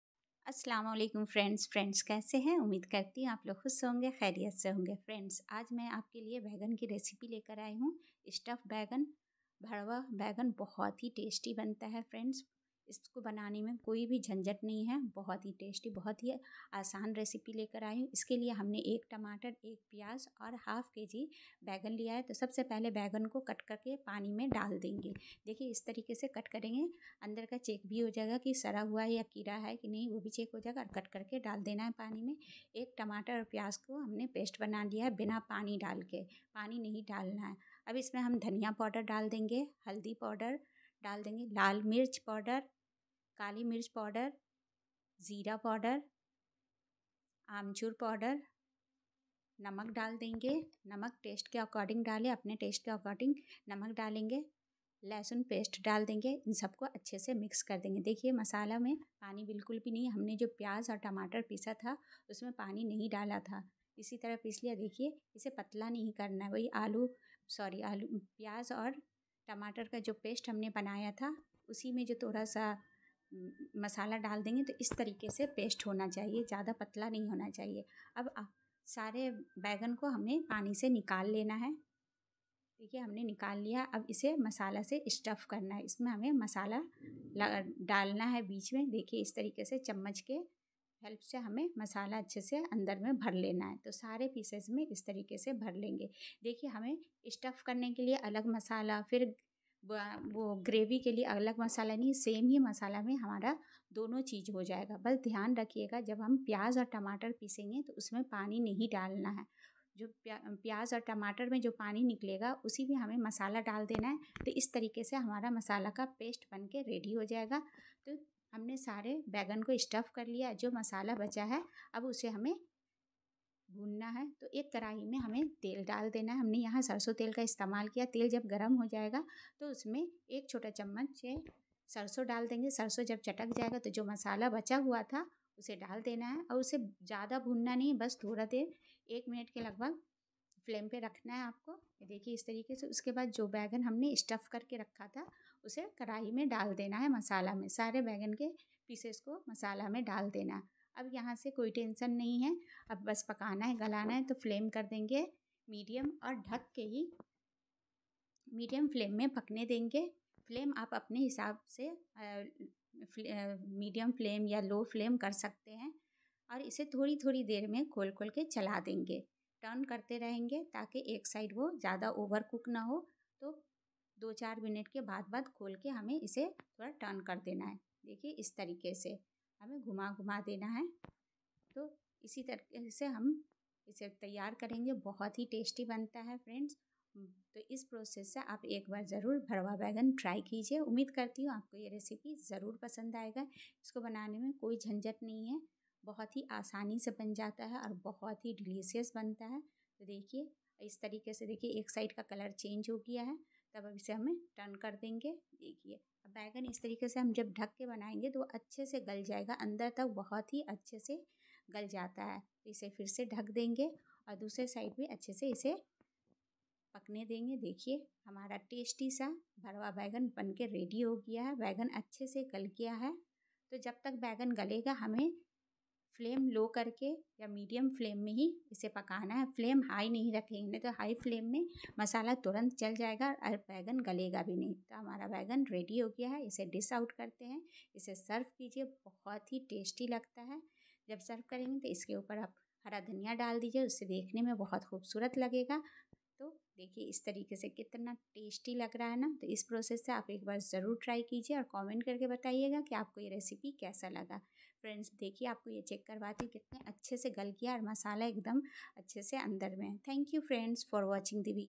सारी बैंगन की सब्जी भूल जाएंगे| Bharwa Baigan | How to make stuffed Brinjal | Simple & easy recipe
सारी बैंगन की सब्जी भूल जाएंगे| Bharwa Baigan & Bajra Roti